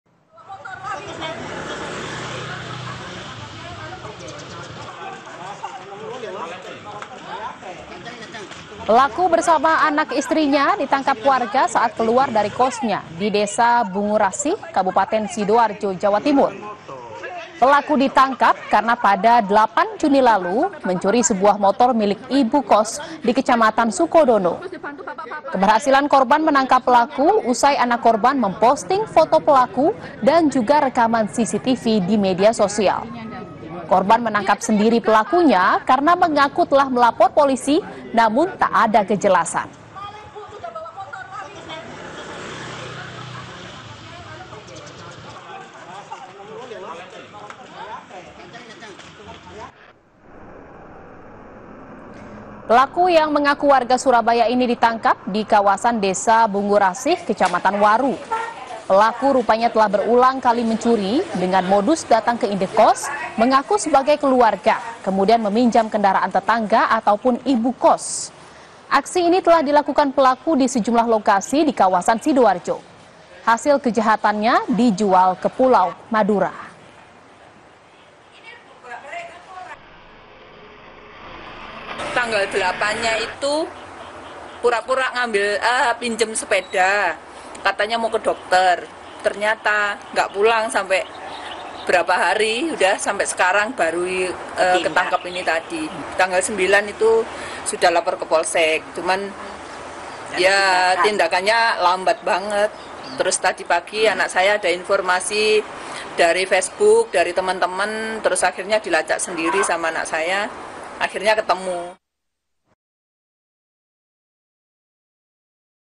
Detik-Detik Korban Emosi Tangkap Sendiri Pelaku Curanmor Gegara Laporan ke Polisi "Diacuhkan"!
Sidoarjo, KompasTV Jawa Timur - Laporan "Diacuhkan" Polisi, Korban Emosi Tangkap Sendiri Pelaku Curanmor di Sidoarjo Jawa Timur!